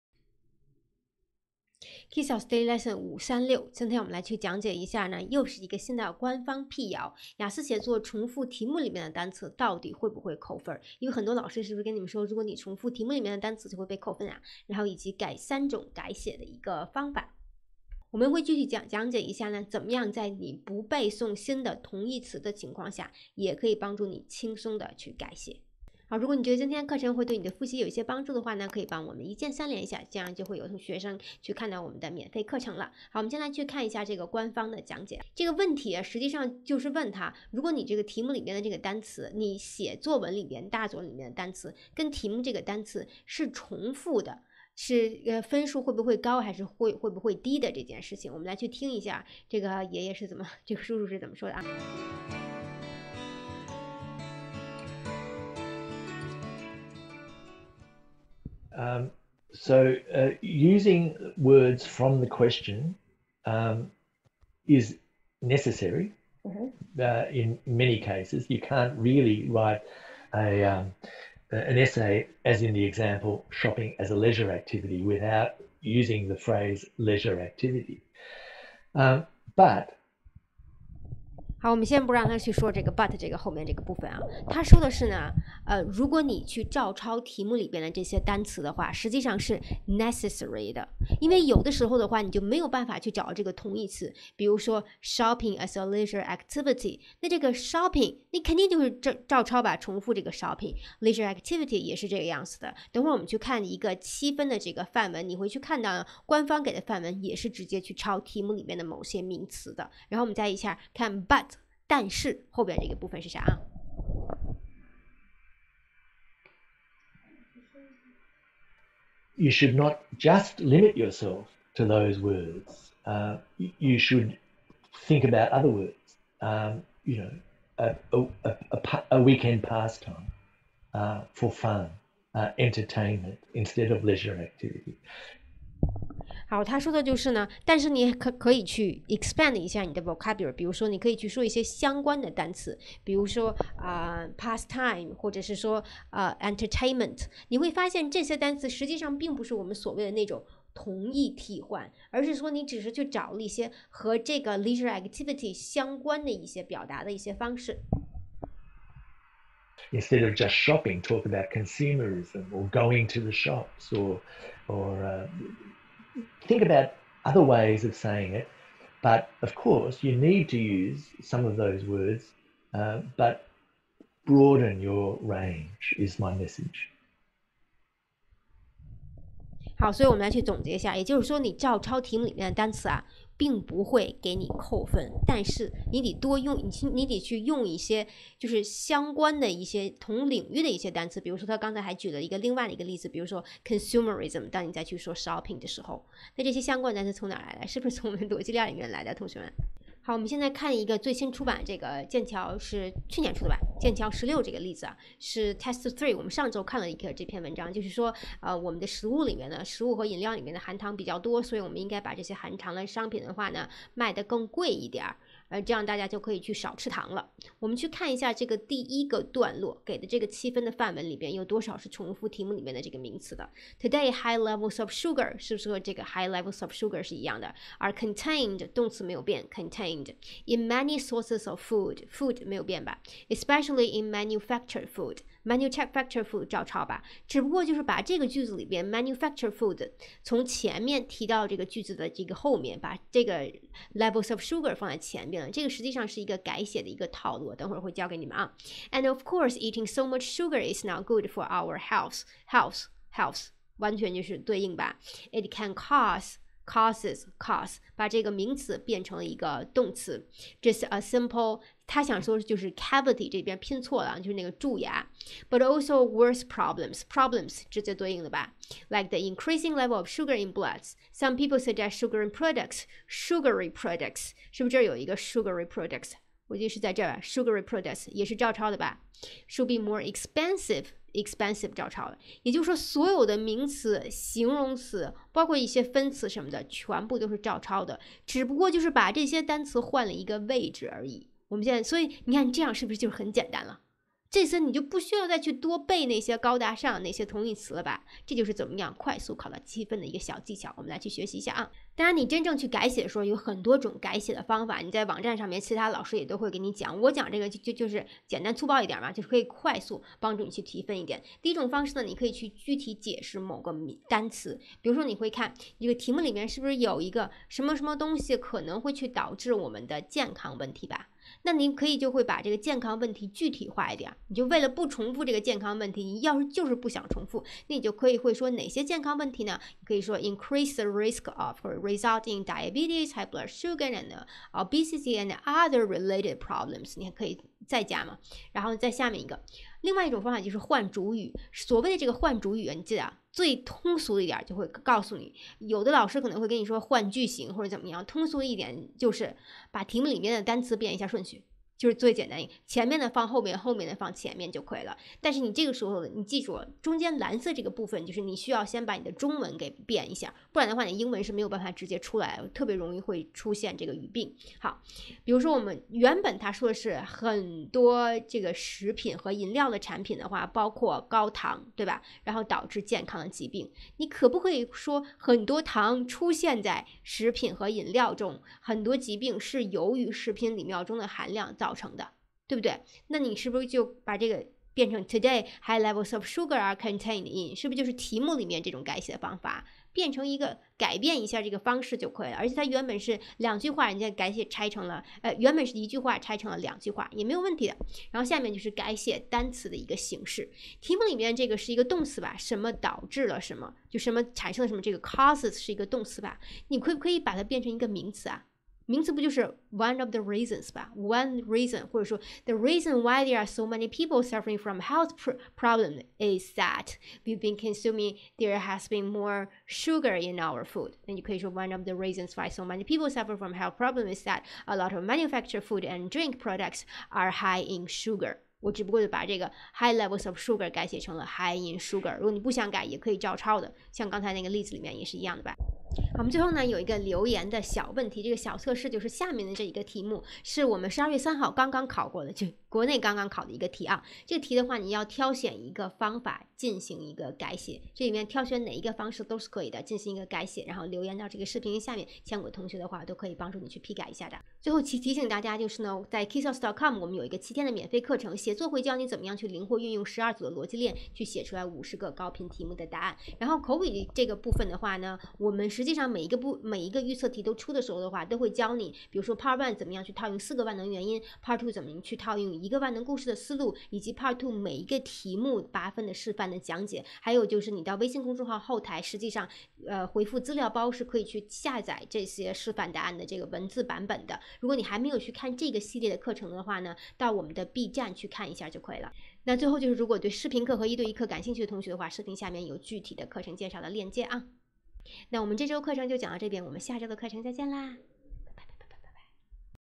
雅思官方辟谣: 雅思写作,重复题中单词,扣分吗？如何改写？L536
雅思写作,重复题中单词,扣分吗？我们应该如何改写?雅思官方辟谣来啦~L536

📘 INDEX-

00:00 本节课概要
00:34 官方讲解
03:23 例子讲解
05:05 三种改写方法
07:17 留言小测试